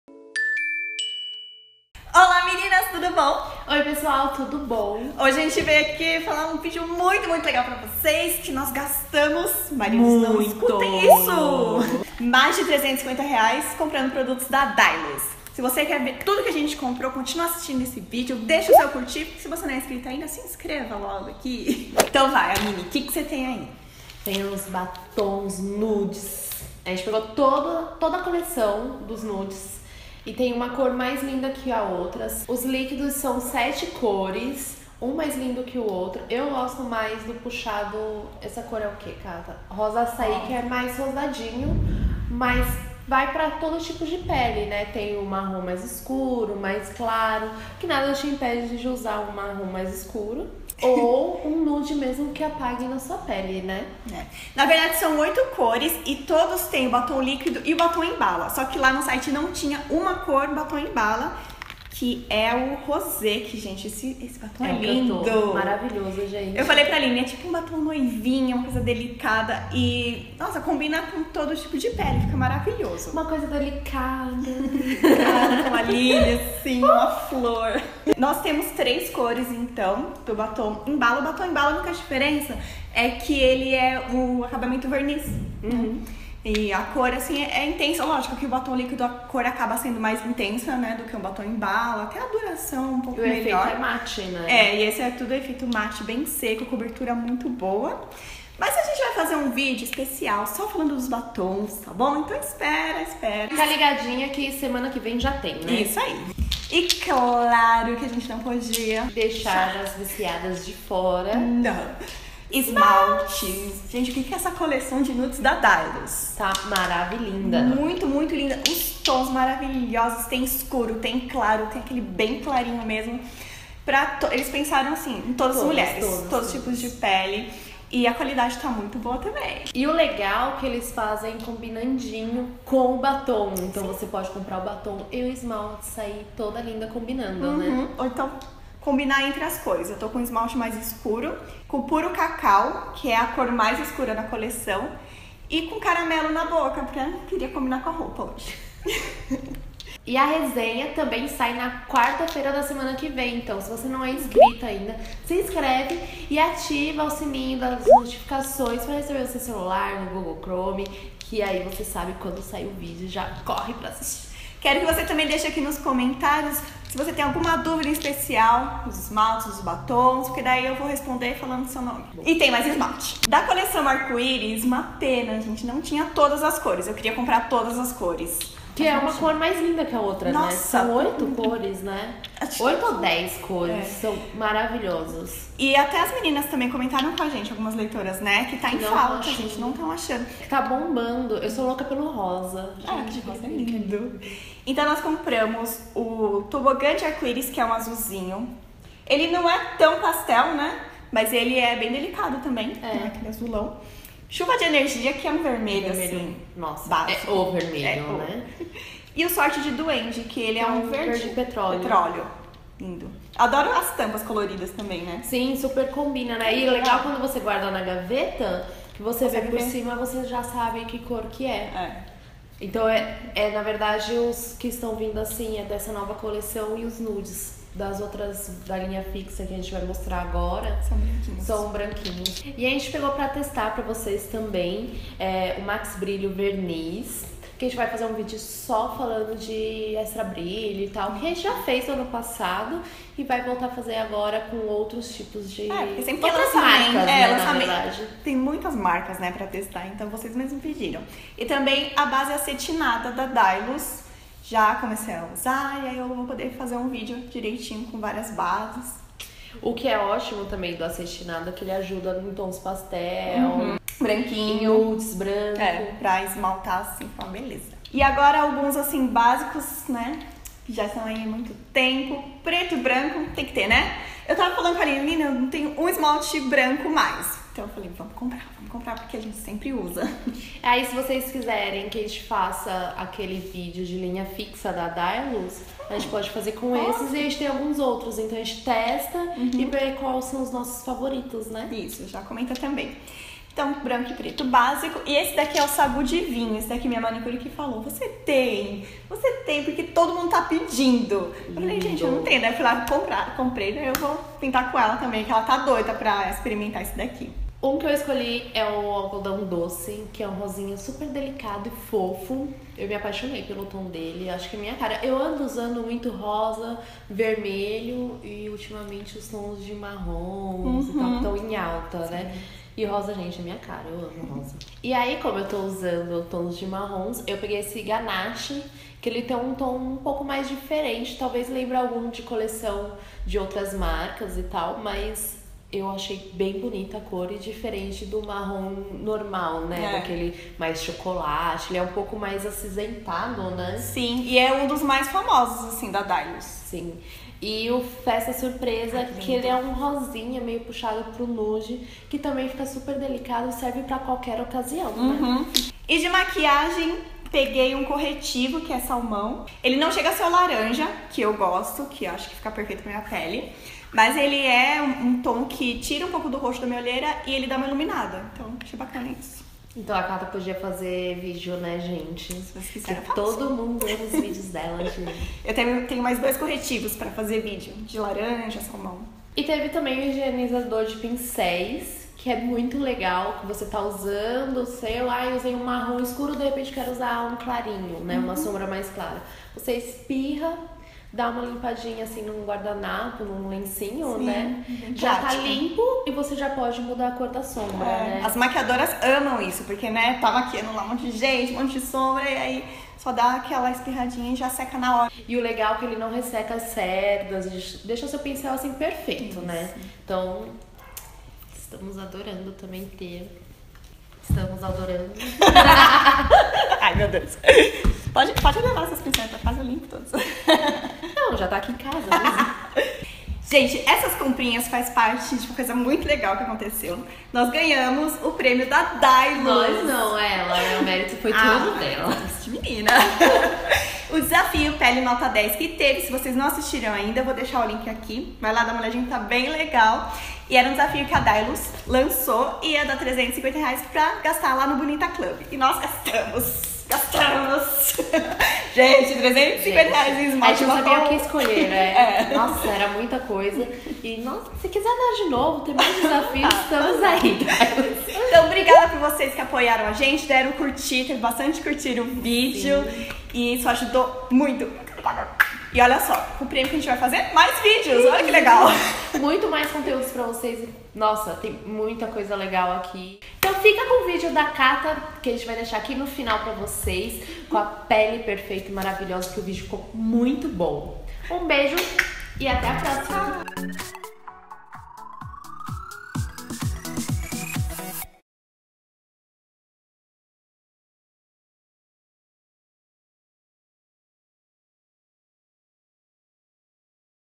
GASTAMOS R$350 na DAILUS 🏆 GANHAMOS DESAFIO DAILUS - Vício de Menina
Queremos agradecer toda a força e likes que vocês deram no vídeo do Desafio Pele Perfeita ♥ Agora vamos aproveitar tudo que compramos para fazer muitos vídeos e posts para vocês.

Nesse vídeo mostramos a "comprinha" de R$350 em produtos Dailus.
Esse valor foi um prêmio que ganhamos no Desafio Pele Nota 10 Dailus. Veja link do vídeo Vencedor

Apoie o Vício de Menina ♥  Ao comprar com nossos parceiros você nos ajuda a investir mais ainda no canal :) Confira abaixo:

♥ Qual Editor vocês usam? ♥ Imovie e Premiere

Músicas